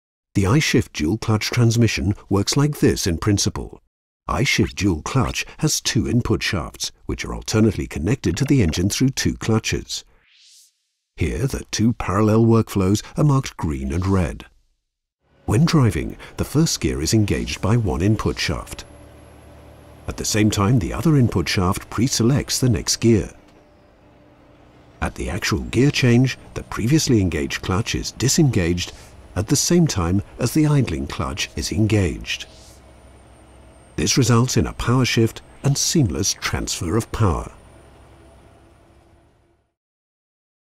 Volvo Trucks - How I-Shift Dual Clutch works
I-Shift Dual Clutch has two input shafts, where one is placed inside the other, and two clutches. With these dual input shafts, two gears can be selected at the same time, with the active gear being determined by which of the two clutches is engaged.